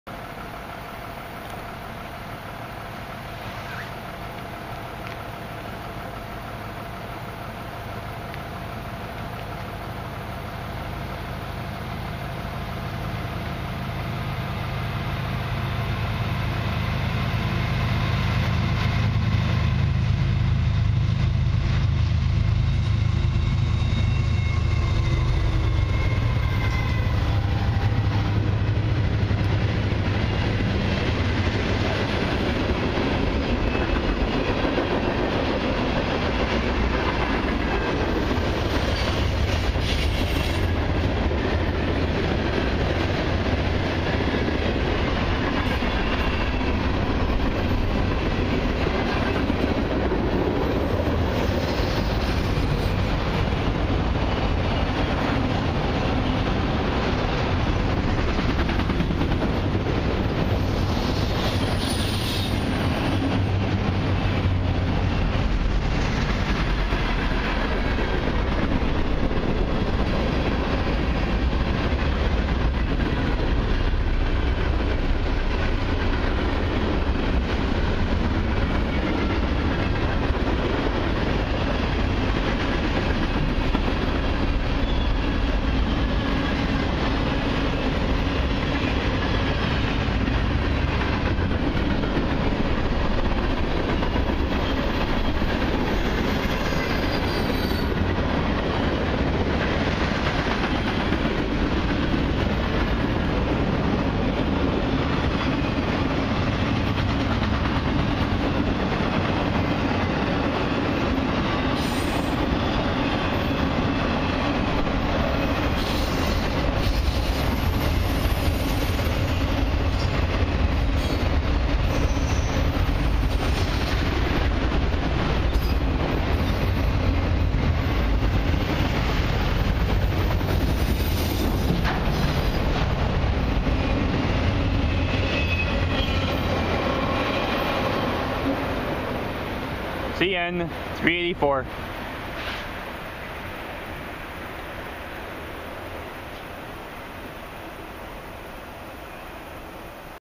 CN 384 w/ CN Engines 2258 & 5619 hooked E.S. lead this long 444 axle, EB Mixed/Autorack train by
.Georgetown GO/VIA Station at the MP of 23 on the CN Halton Sub.

Even tho I was here the previous Saturday, once again I headed off to Georgetown myself via the GO bus back on December 9th. Met my friend Larry there as well.

So, here's my 1st catch of 7 from here at Georgetown/Mt. Pleasant on my way home from back on December 9th.

video taken: Saturday December 9th 2023 at 10:50am